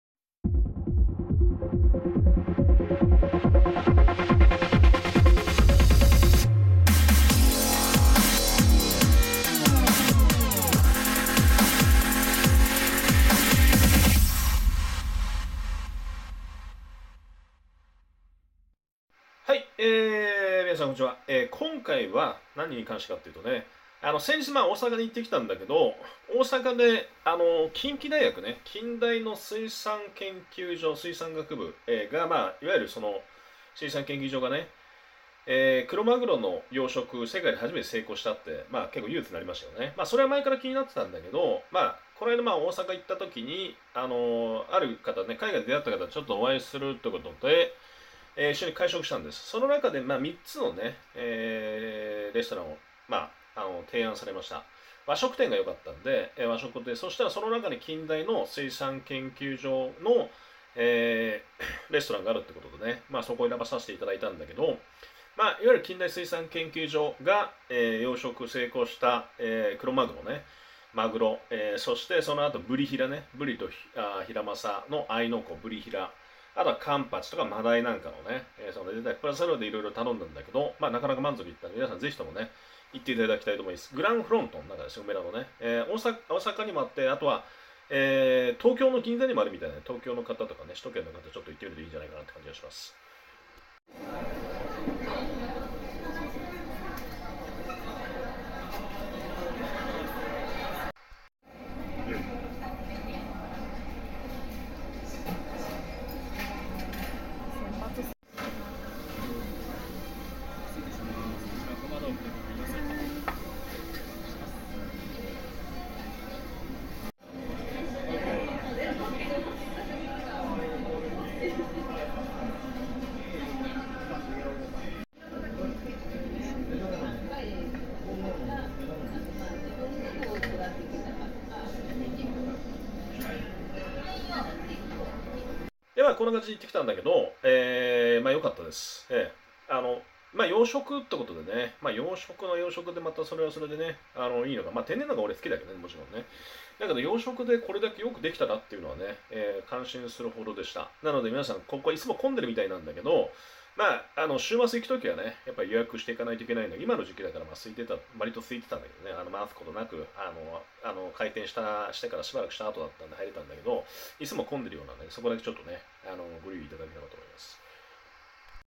大阪梅田グランフロント！近畿大学水産研究所にて世界初養殖クロマグロを中心とした海鮮料理堪能！ Japanese Seafood,kindai suisan kenkyujo,Osaka ,Japan
【大阪梅田・近畿大学水産研究所にて世界初養殖クロマグロを中心とした海鮮料理堪能】
I ate seafood at kindai suisan kenkyujo in Umeda granfront in Osaka last night.

I ate the farmed fish such as bluefin tuna and the natural fish which Kinki University Fisheries Research Institute succeeded for the first time in the world.

昨晩は、大阪梅田のグランフロント大阪北館6Fにある近畿大学水産研究所にて夕食。世界初のクロマグロ養殖に成功した近大水産学部は前々から気になってはいましたが、それら養殖魚を味わえるレストランが梅田と銀座の2店舗あります。

近大水産学部が養殖した魚を中心に、天然魚も交えた品々を注文できます。

オススメは養殖のクロマグロの中トロやマダイ、ブリとヒラマサ組み合わせたブリヒラ、カンパチなどの刺身盛り合わせ。

他にもあら汁やアヒージョ、だし巻き玉子などを注文。

いつもは非常に混雑しているようで、土日は待ちが当たり前のようですが、コロナの影響とわりと早めに入店したのもあり、待つことなく着席できました。

釣りが好きな自分ですが、水産系や海洋系の学問も面白そうですね。